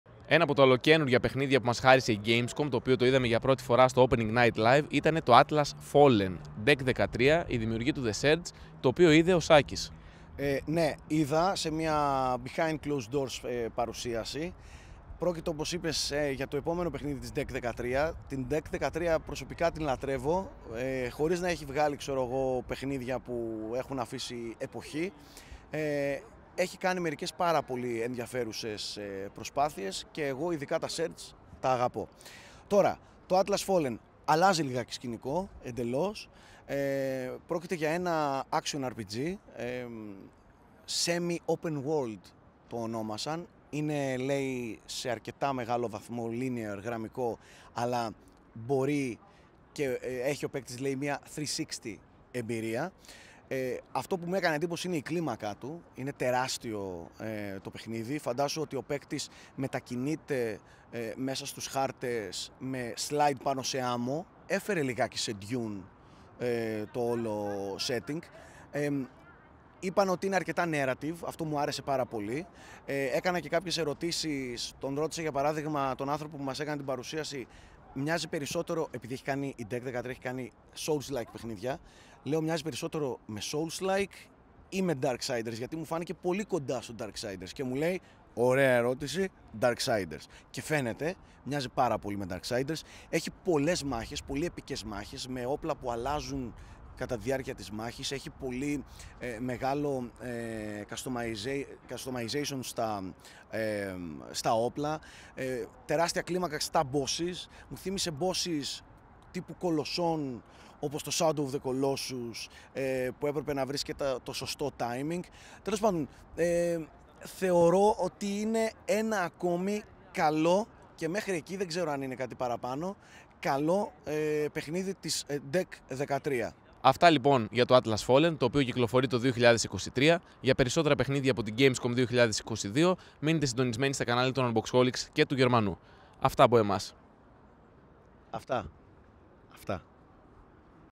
Atlas Fallen - Review by Unboxholics - Gamescom 2022 | GERMANOS
Οι @Unboxholics έπαιξαν το ολοκαίνουριο "Atlas Fallen". Ο Σάκης Καρπάς απόλαυσε το action RPG game το οποίο διαδραματίζεται σε semi-open world με 360 εμπειρία του χρήστη.

Διαβάστε όλα τα νέα της Gamescom 2022 και στο G Blog